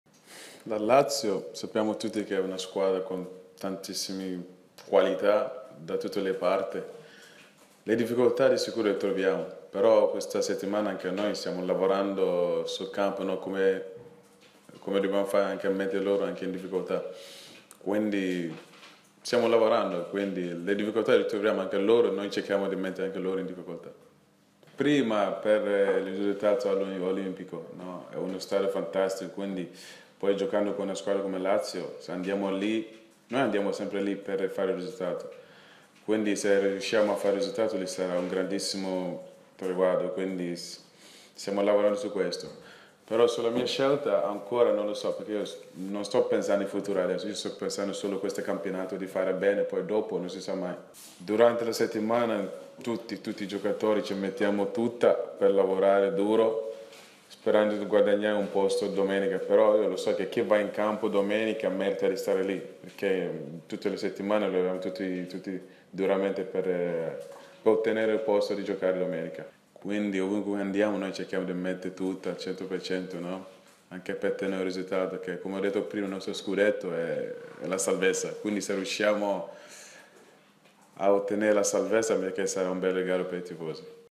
Chibsah: "Lazio, missione risultato" - Serie A TIM 2015/16
Il ghanese spavaldo prima della trasferta di Roma